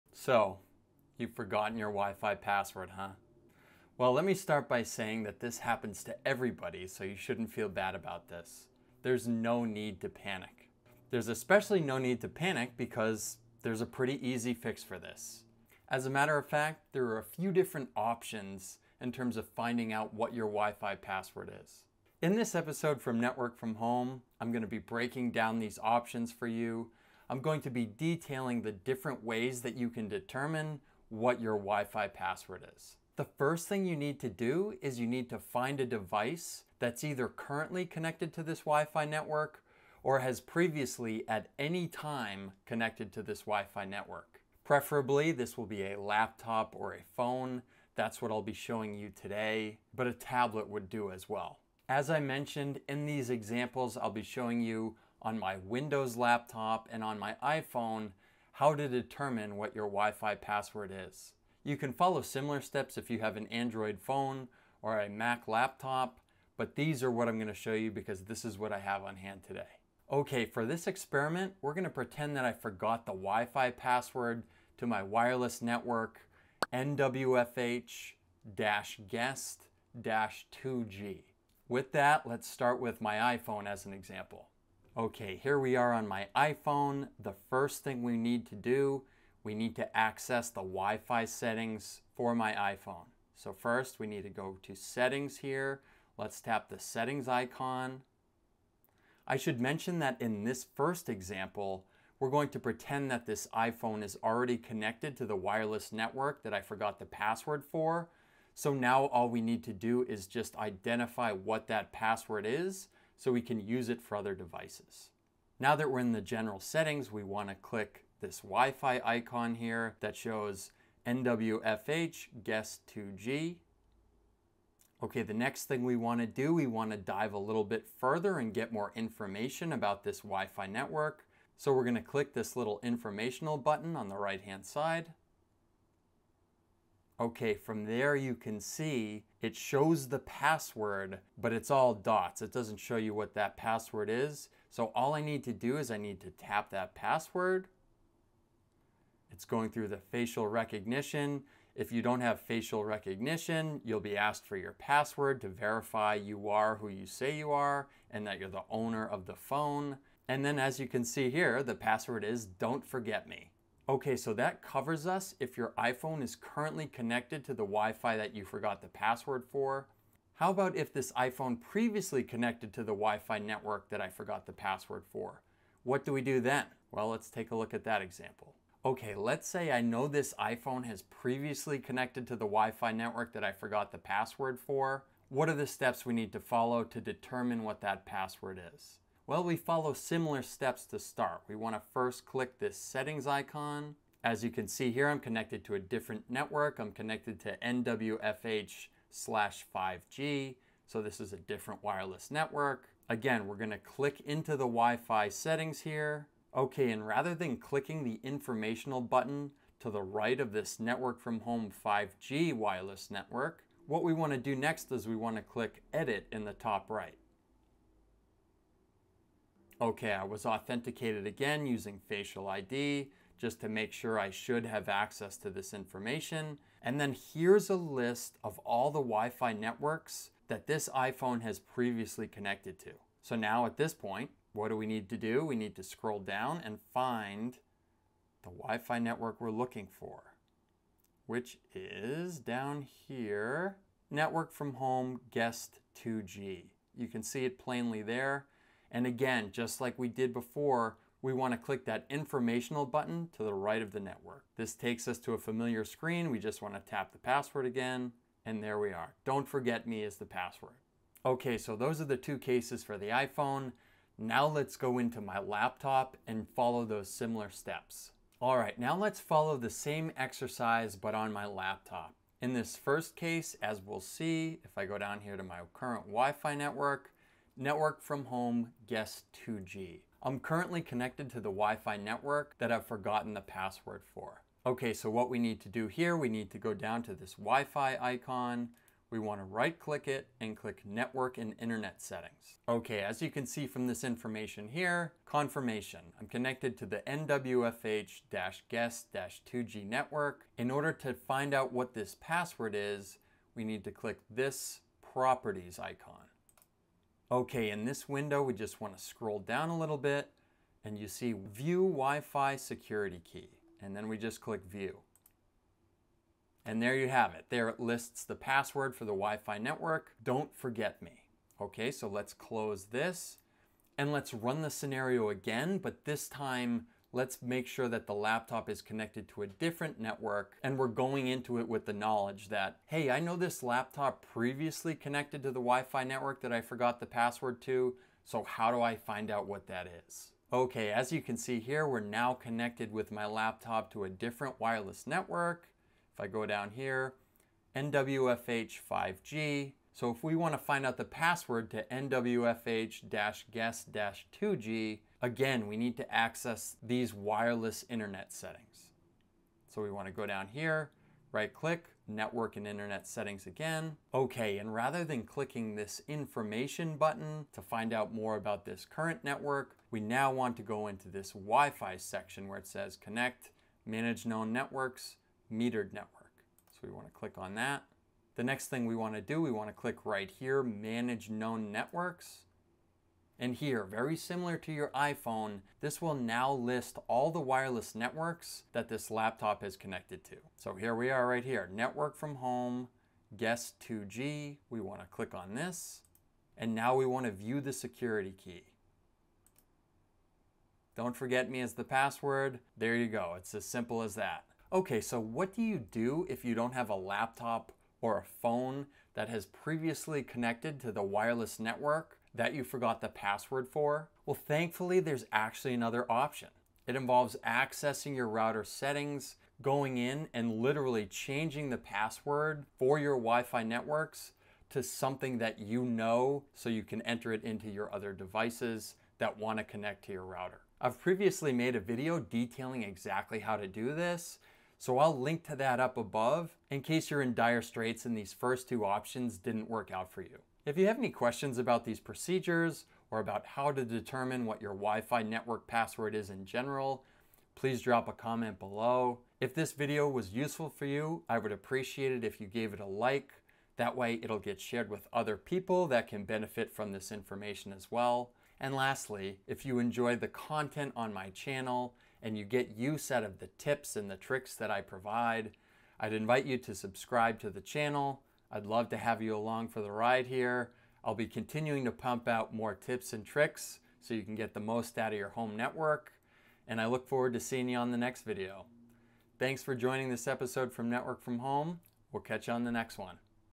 What to Do if You’ve Forgotten Your WiFi Password - EASY FIX
In this video, I provide a few different options for determining the password of your WiFi network if you’ve forgotten it…

0:00 Introduction
0:33 What you need to determine your WiFi password
1:24 How to determine your WiFi password with an iPhone
4:28 How to determine your WiFi password with a Windows 11 laptop
6:44 Another alternative for determining your WiFi password
7:20 Summary and conclusion

What to do if you’ve forgotten the password for your WiFi network
If you’ve forgotten the password to your WiFi network, thankfully you have a few options.

The first thing you need to do is find a mobile device (laptop, phone, or tablet) that:
--Is currently connected to the WiFi network you forgot the password to

OR

--Was previously connected to the WiFi network you forgot the password to

As long as you have a mobile device that has connected to the WiFi network that you forgot the password to at least once, you should be able to use the device to look up the network’s password.

That’s because these mobile devices maintain a list of all the WiFi networks they’ve previously connected to, along with the password for each network. You just need to know where to look.

If you don’t have a mobile device that has connected to the WiFi network that you forgot the password for, all is not lost.

You have the option of using a device to log in to your router’s settings to change your WiFi network password to something that you won’t forget.

If this is the situation you have found yourself in, I have previously made a video detailing how you can change the password of your WiFi network.

With all these options at your disposal for recovering your WiFi password or creating a new one, you should be back up and running in no time.